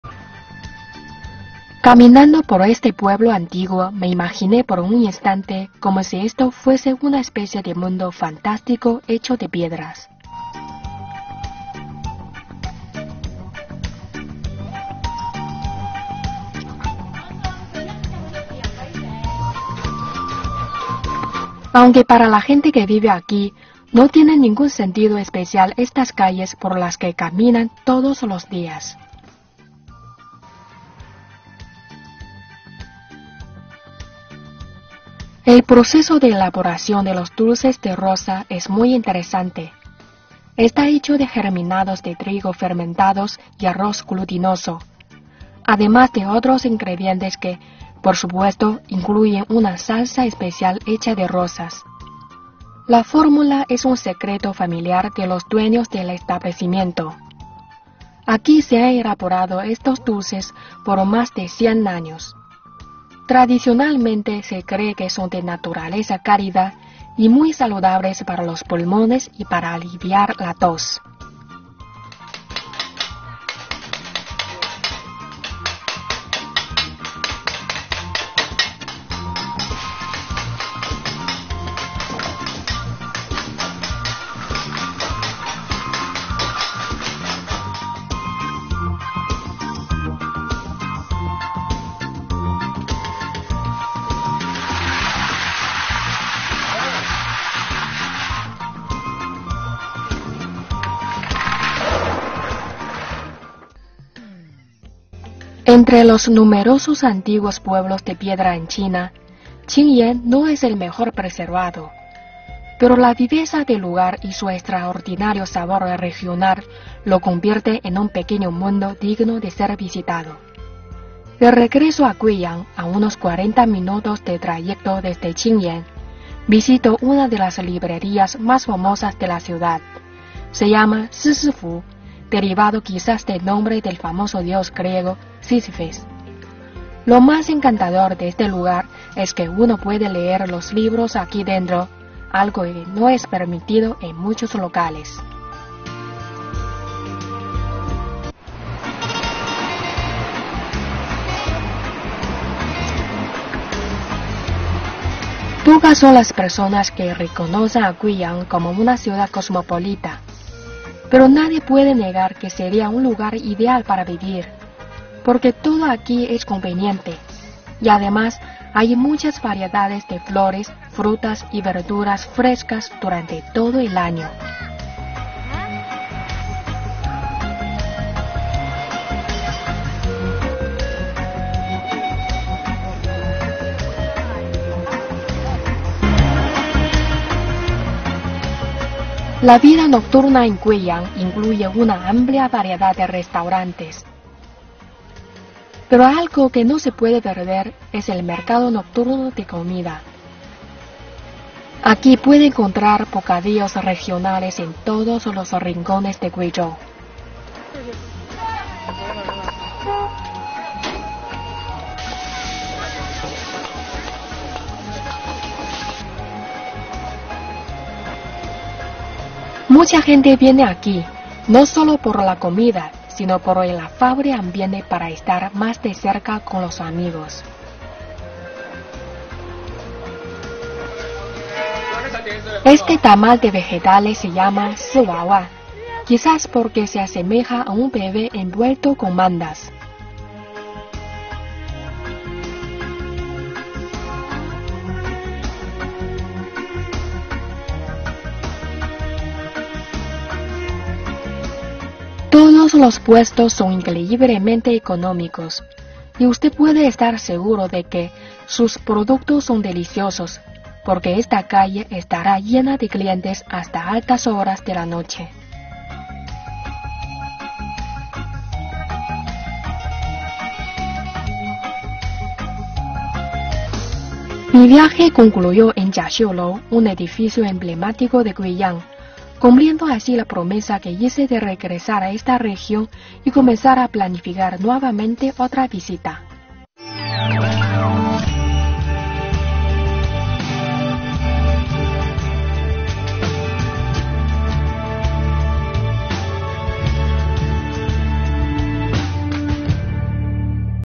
DIARIOS DE VIAJE (travel journal) 09 28 2015 Oeste de Guizhou 0006
It is an intertesting video travel journal record the culture, humanity and life of Guizhou.

Part 6